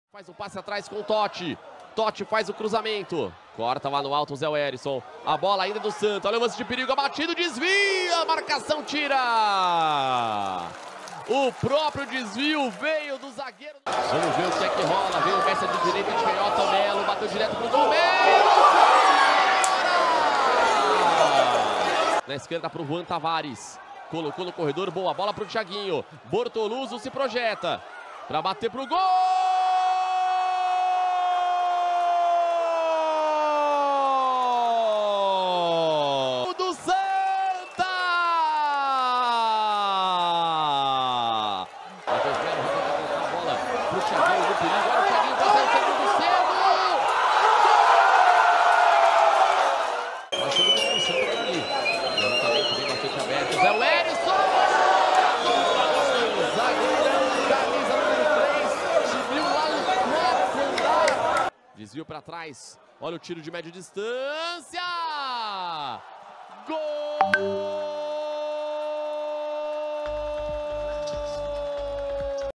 Porto-PE 1 x 3 Santa Cruz - Pernambucano 2024
31/01/2024
Campeonato Pernambucano - 1ª Fase
Porto-PE 1 x 3 Santa Cruz
Gols: Pedro Bortoluzo 30 do 1º; Thiaguinho 1, José Werison 7 e Caio Mello 18 do 2º

Créditos: Canal GOAT